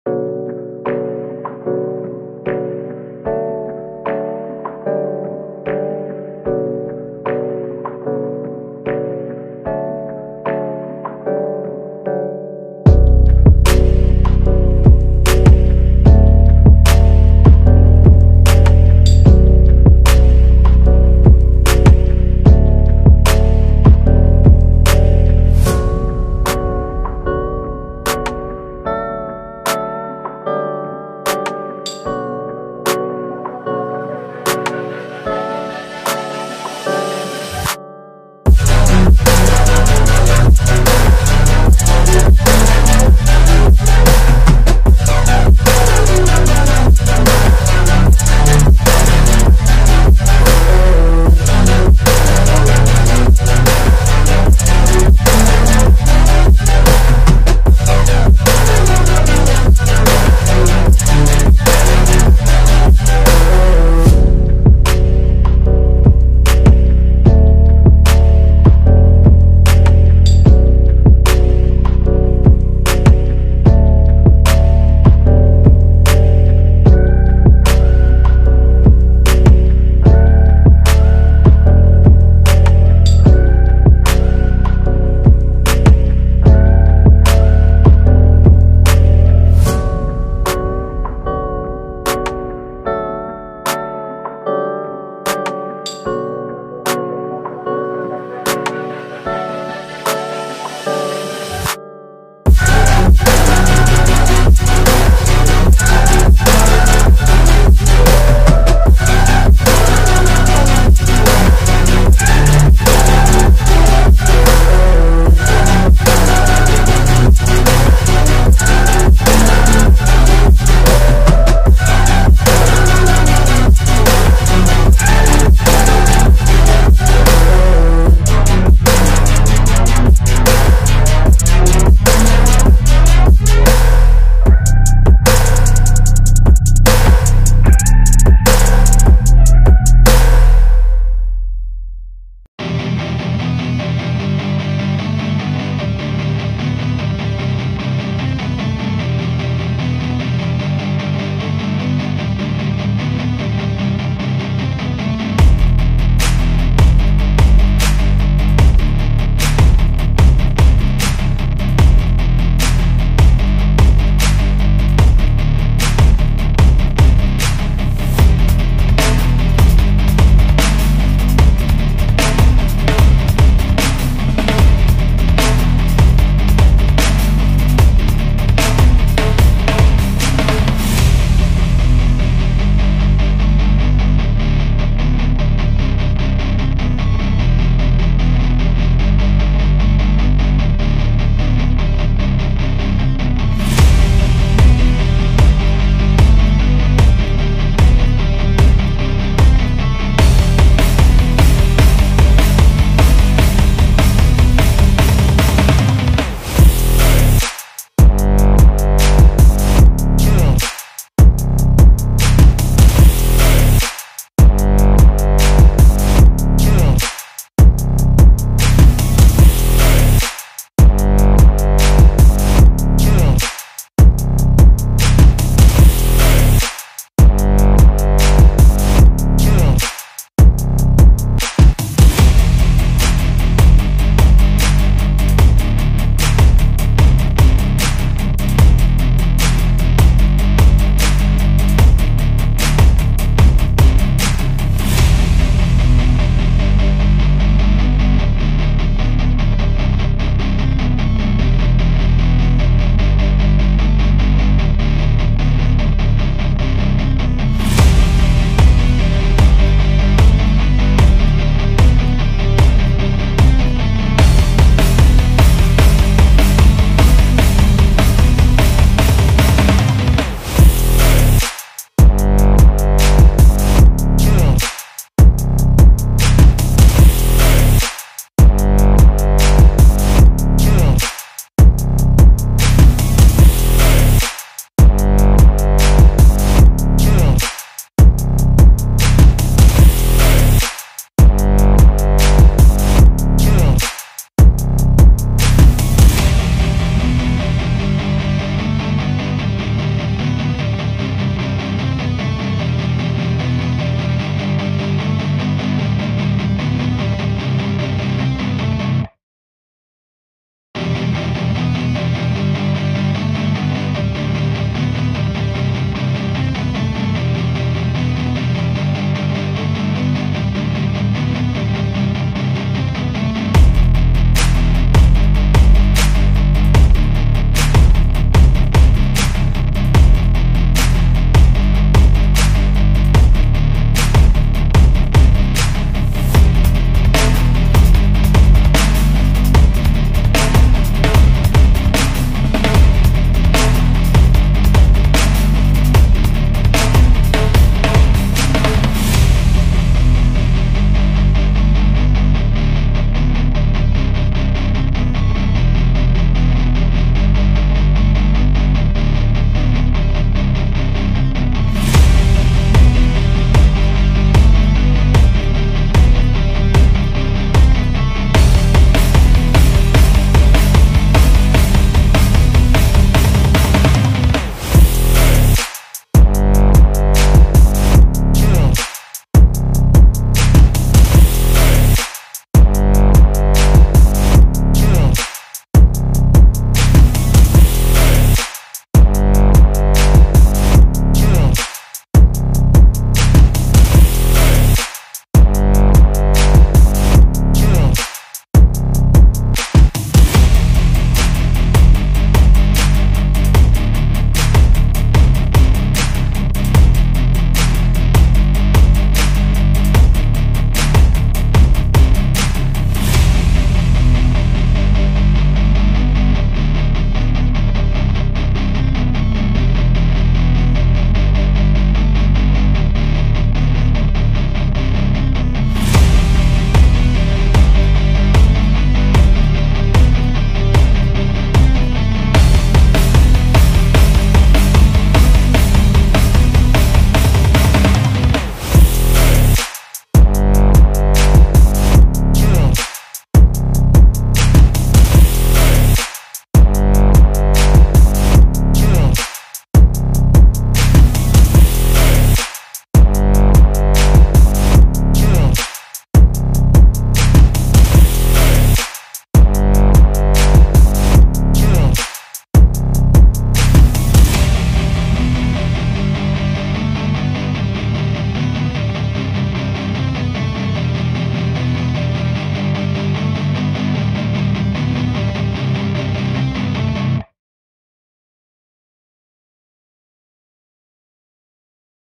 USA vs Panama 5-1 Hіghlіghts & Goals - WC Qualifying
C-pulisic bang another hattrick against Panama